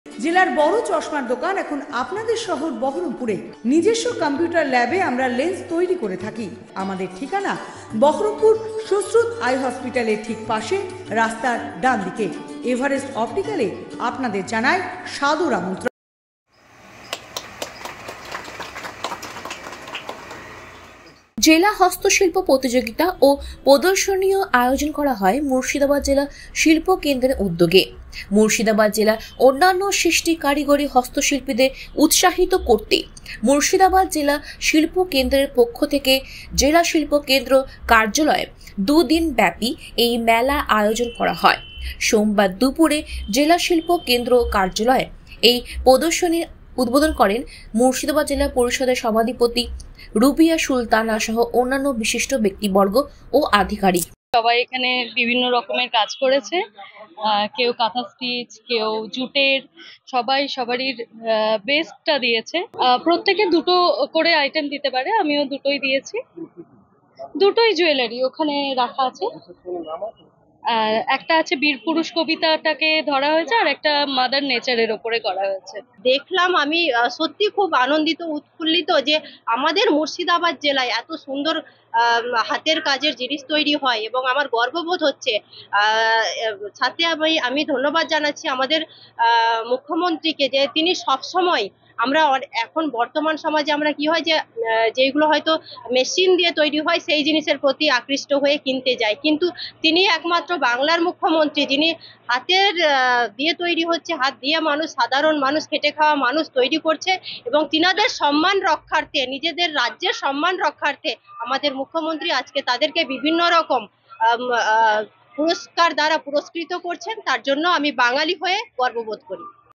Berhampore: জেলা হস্তশিল্প প্রতিযোগিতা ও প্রদর্শনী মেলা বহরমপুরে
হস্তশিল্পীদের উৎসাহিত করতে মুর্শিদাবাদ জেলা শিল্প কেন্দ্রের পক্ষ থেকে জেলা শিল্প কেন্দ্র কার্যালয়ে 2 দিনব্যাপী এই মেলার আয়োজন করা হয়।